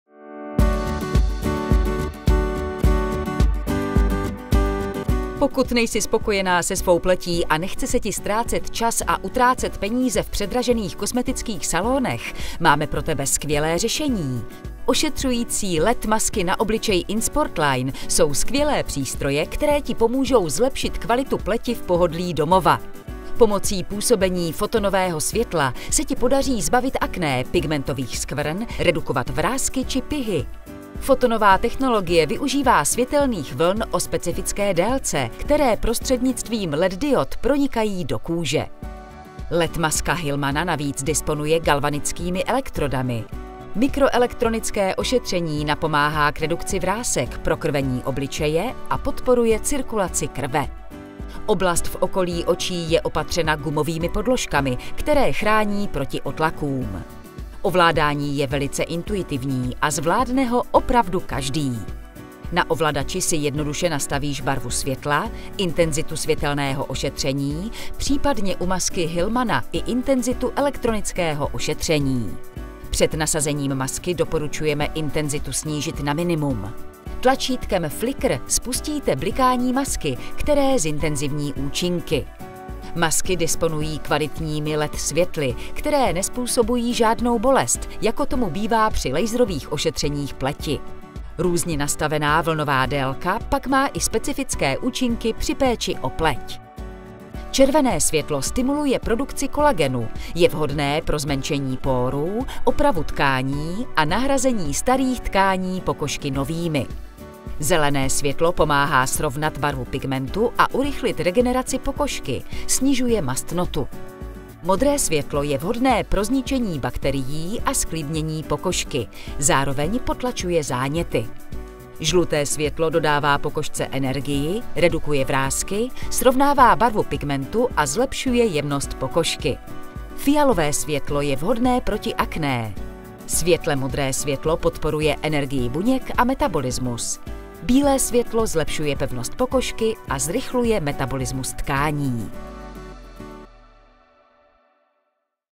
LED masky inSPORTline
Pokiaľ nie si spokojná so svojou pleťou a nechce sa ti strácať čas a utrácať peniaze v predražených kozmetických salónoch, máme pre teba skvelé riešenie! Ošetrujúce LED masky na tvár inSPORTline sú skvelé prístroje, ktoré ti pomôžu zlepšiť kvalitu pleti v pohodlí domova! Pomocou pôsobenia fotónového svetla sa ti podarí zbaviť akné, pigmentových škvŕn, redukovať vrásky či pehy. Fotónová technológia využíva svetelné vlny o špecifickej dĺžke, ktoré prostredníctvom LED diód prenikajú do kože.
LED maska ​​Hilmana navyše disponuje galvanickými elektródami. Mikroelektronické ošetrenie napomáha k redukcii vrások, prekrveniu tváre a podporuje cirkuláciu krvi. Oblasť v okolí očí je opatrená gumovými podložkami, ktoré chránia proti otlakom. Ovládanie je veľmi intuitívne a zvládne ho naozaj každý!
Na ovládači si jednoducho nastavíte farbu svetla, intenzitu svetelného ošetrenia, prípadne pri maske Hilmana aj intenzitu elektronického ošetrenia. Pred nasadením masky odporúčame intenzitu znížiť na minimum. Tlačidlom Flicker spustíte blikanie masky, ktoré zintenzívnia účinky.
Masky disponujú kvalitnými LED svetlami, ktoré nespôsobujú žiadnu bolesť, ako tomu býva pri laserových ošetreniach pleti. Rôzne nastavená vlnová dĺžka potom má aj špecifické účinky pri starostlivosti o pleť.